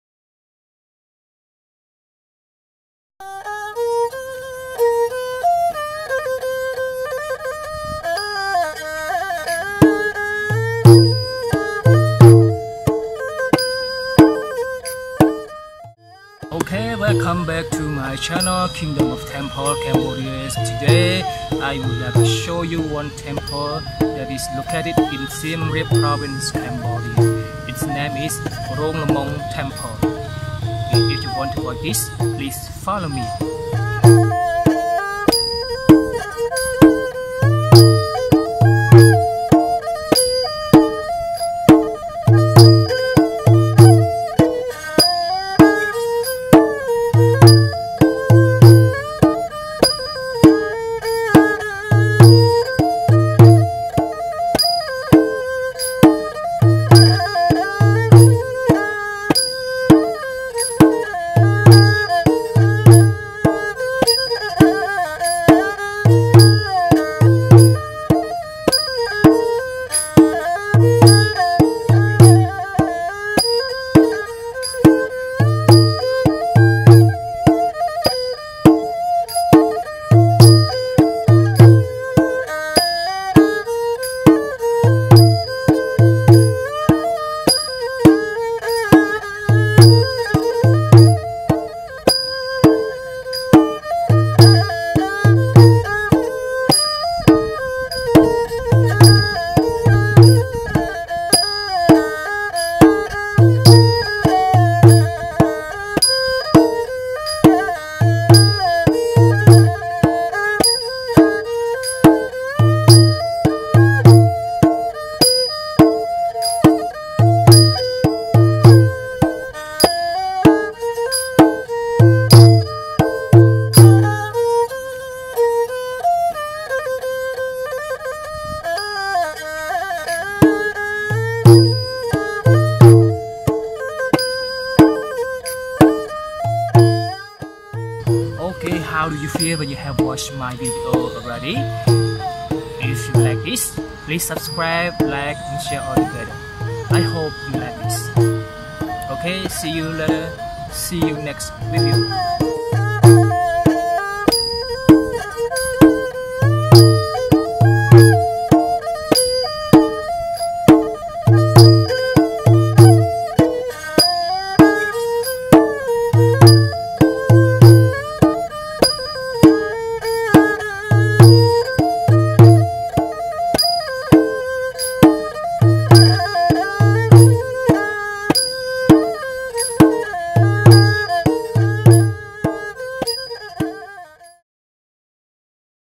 Rong Lmung Temple | Kingdom of Wonder | Siem Reap | Welcome to Cambodia
Welcome to Cambodia
Rong Lmung is one of the small brick temples, located in Siem Reap Province, Cambodia.
I'm going to take all of you to visit my country, the Kingdom of Wonder, Cambodia that fulls of many temples like Angkor Wat, Bayon, Banteay Srey Temples